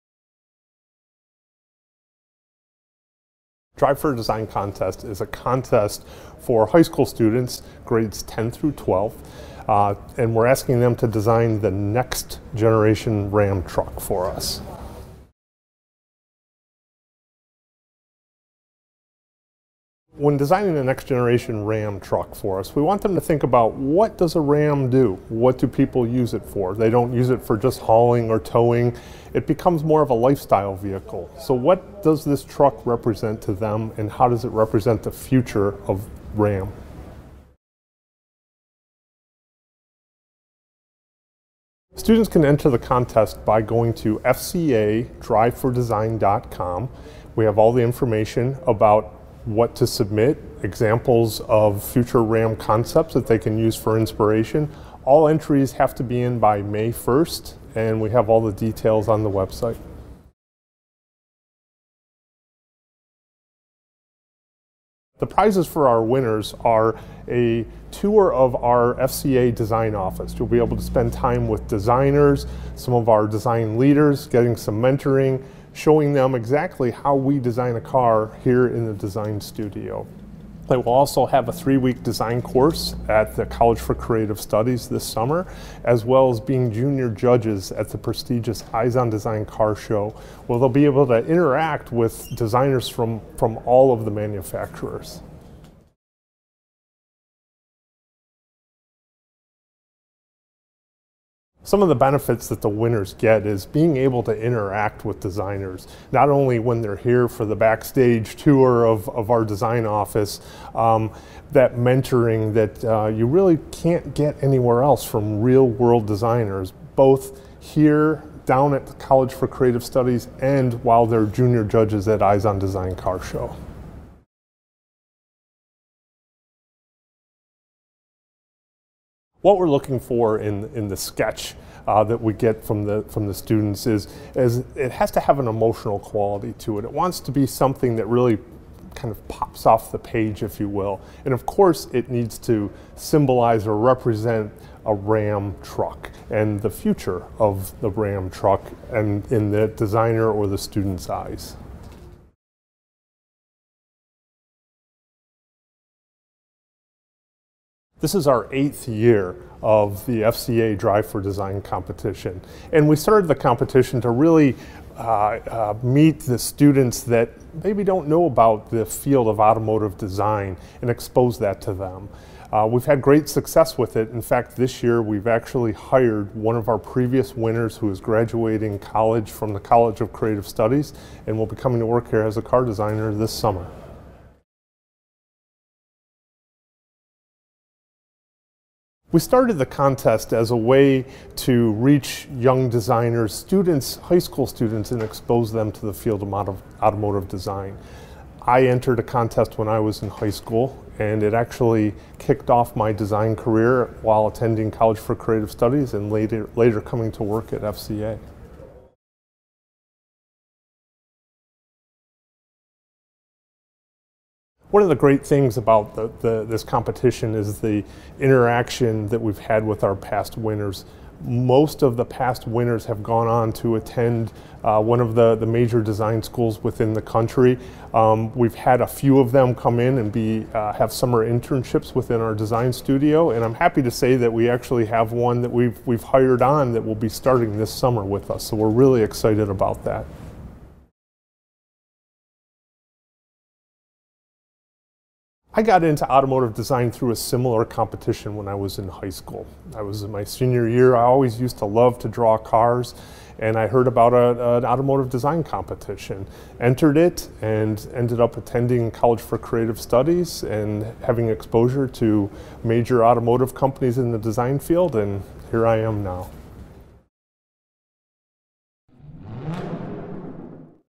Next Generation Ram Truck Design
Without a nudge from an art teacher, a visit to an art school or just plain luck, some of the world’s top automotive designers may never have made their mark on history.

Mark Trostle, Head of Ram Truck Design at FCA, wants to change that. Eight years ago, he revived a contest he won as a high school student that helped drive him to where he is today, leading the design efforts for some of the most desired vehicles on the road.

The 2020 Drive for Design contest challenges U.S. high school students in grades 10-12 to sketch a Ram truck of the future. And, just like in the professional world, there is a level of urgency – entries are due by May 1, 2020. Three students will be named winners from all valid entries received.

“The landscape of automotive design has changed and today’s automotive designers not only sketch cars, they create some of the most advanced user experience systems, work with 3D technology, research color trends and create interior spaces that are multifunctional and beautiful,” said Trostle. “As the field of automotive design grows, it’s vital that we increase awareness and educate students and parents about the career opportunities available.”

The Drive for Design competition kicked off during the EyesOn Design commemorative poster unveiling held at the Detroit Institute of Ophthalmology in Grosse Pointe Park, Mich.